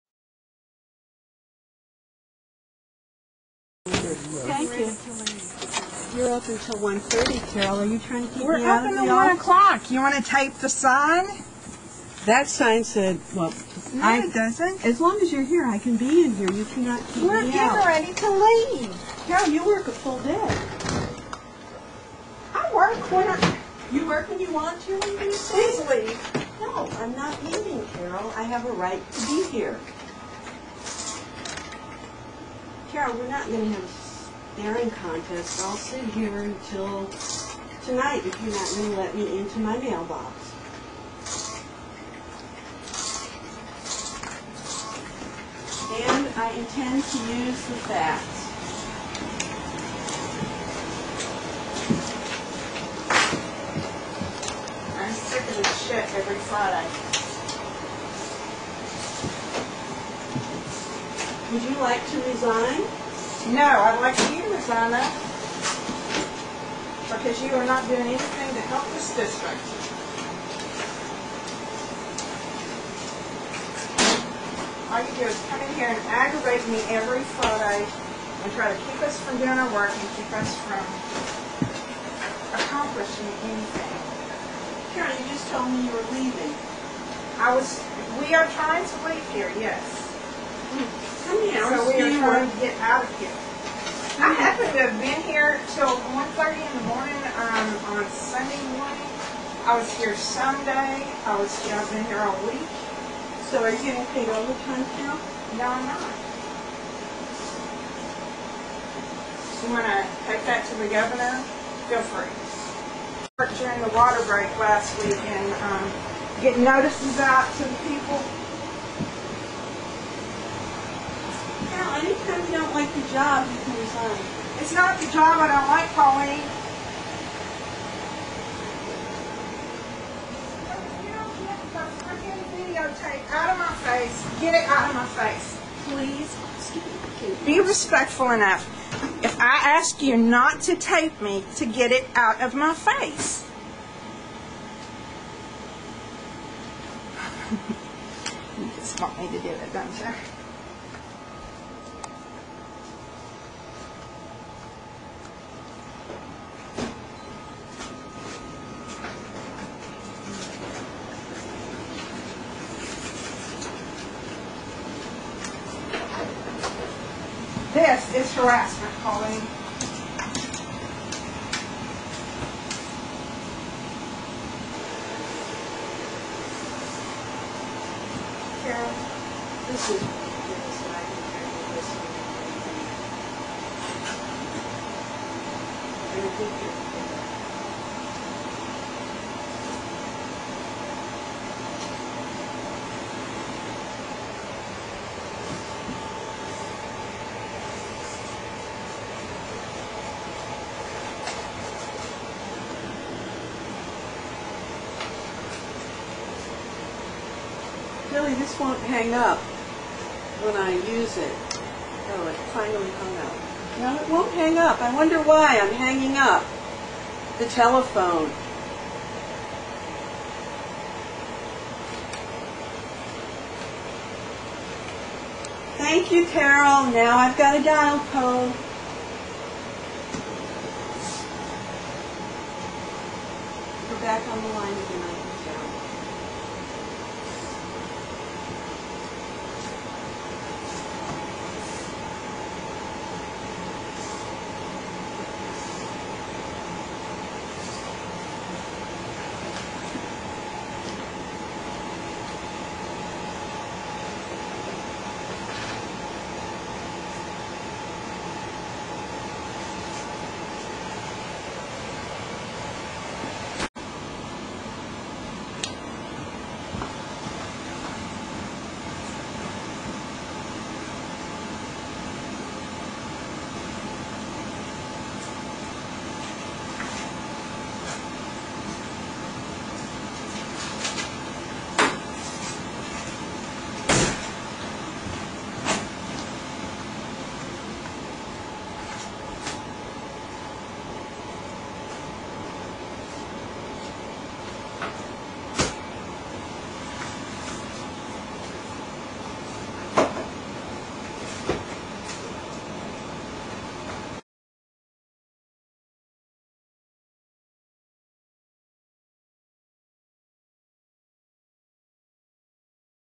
Reynolds Playing Games in the Sewerhood
This video segment shows how welcoming the Lanark Village Water and Sewer District is of its newest commissioners.  Ms. Reynolds has an unusual professional decorum and seems to like staring contests, four letter words, and telephone games.
On March 8, 2007 Attorney Brian Armstrong spoke with Barbara Rohrs about new commissioner bonding issue.  He also spoke with Rohrs about other issues related to commissioner status on April 20.  On May 9, 2007 Amrstrong reviewed an issue concerning prohibition of the dissolution of LVWSD because of federal government loans.  On May 28, 2007 Armstrong and Rohrs talked about the issue of mandatory monthly meetings.  Come to the August 20, 2007 Lanark Village Water and Sewer meeting to see how much money is paid for Rohrs, Thoman, and Reynolds to speak with Brian Armstrong.
Audience Questions:
Who taught Carol Reynolds these games?  Did LVWS&D pay for these lessons?  If so, did we get our money's worth?